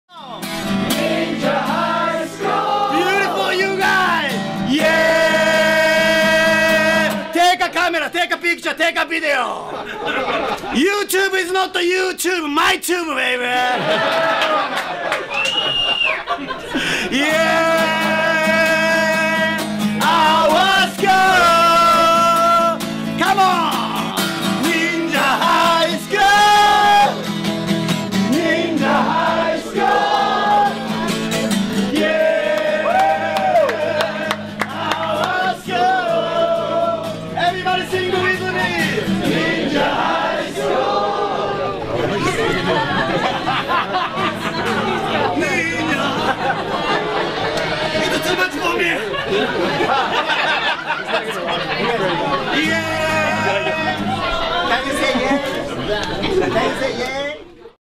Spinning Shell Presents Peelander Yellow ArtShow
Kengo Hioki also known as Peelander Yellow! The singer/ guitarist of Japanese action comic punk band Peelander-Z showcasing his art at Guzu Gallery on July 13th